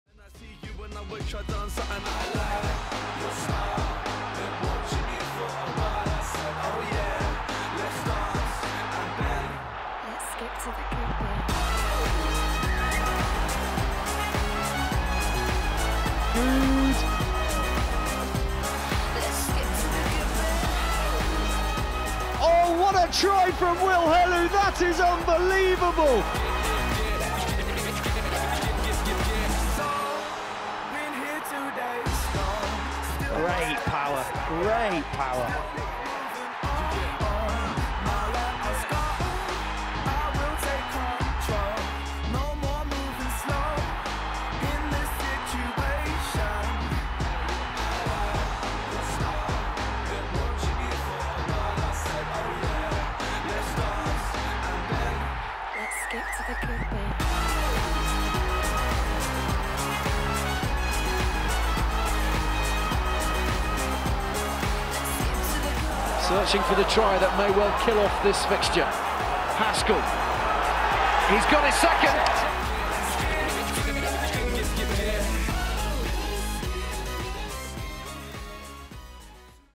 Players' Player of the Season
All the top four nominees for the Players' Player of the Season 2013/14.

The award, voted for by London Wasps players, was shared between James Haskell and Will Helu.